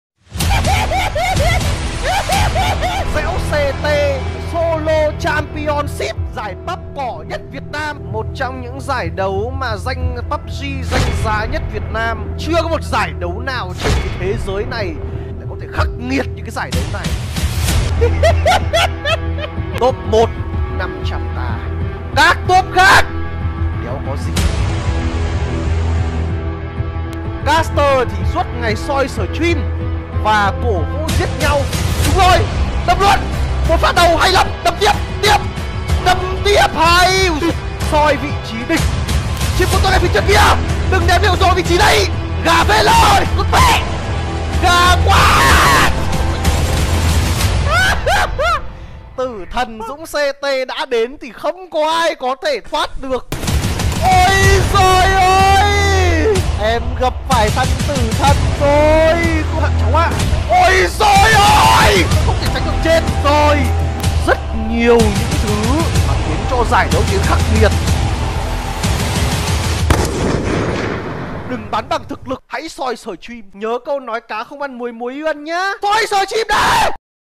Trailer Giải Đấu Cỏ 2019: "DCT Solo Championship"
DCT Solo Championship Là Giải Đấu Cỏ Theo Thể Thức TPP Solo Được Trực Tiếp Game Tổ Chức Và Do Dũng CT Làm Caster Với Phần Thưởng 500k Mỗi Round Đấu Giúp Cho Các Anh Em Cter Có Cơ Hội Được Thể Hiện Và Cọ Xát Với Nhau.
Edit By Thanh Vương (Techman Nguyen)